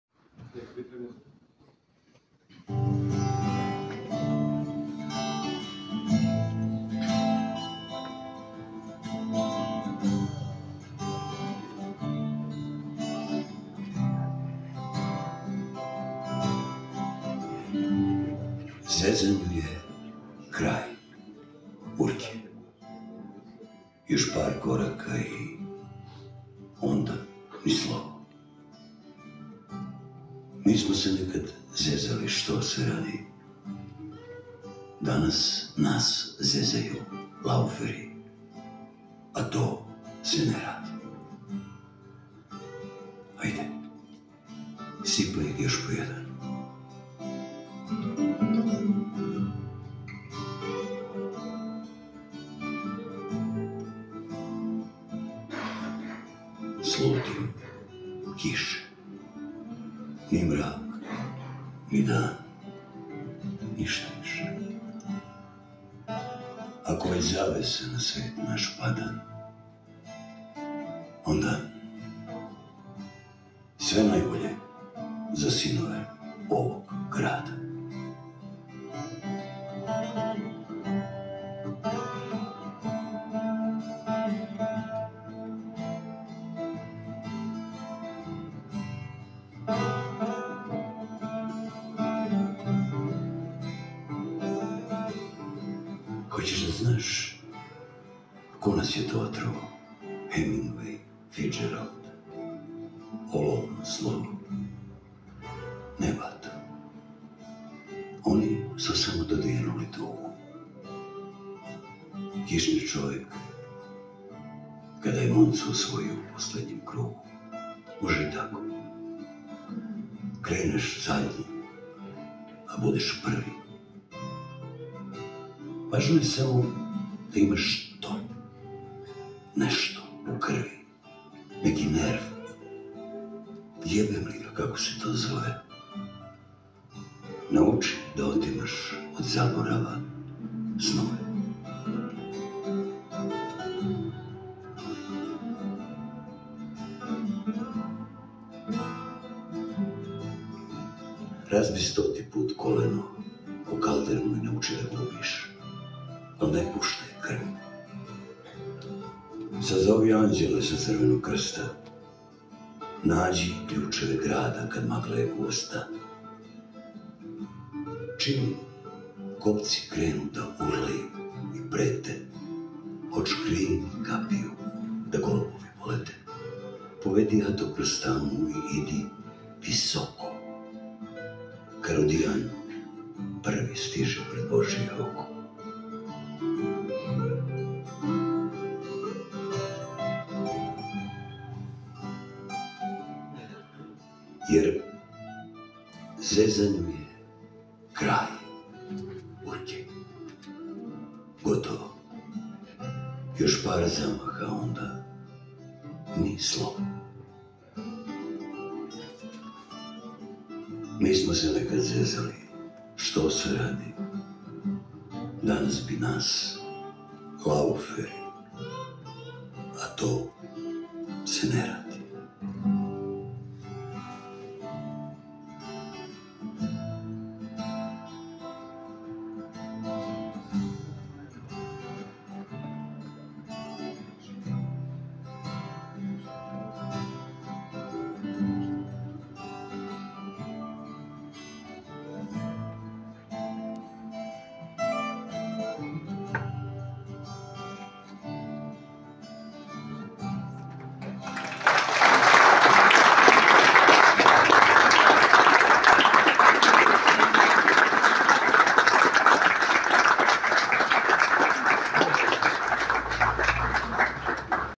GAGA-sanjao sam
Dragan Gaga Nikolić, rođen je u Beogradu pre osamdeset godina (1943 – 2016), a u nedelju, 24.decembra 2023 u 18.30, u Ateljeu 212, njegovoj nekadašnjoj matičnoj kući, biće predstavljen roman „GAGA-sanjao sam“, autora Dušana Stojanovića.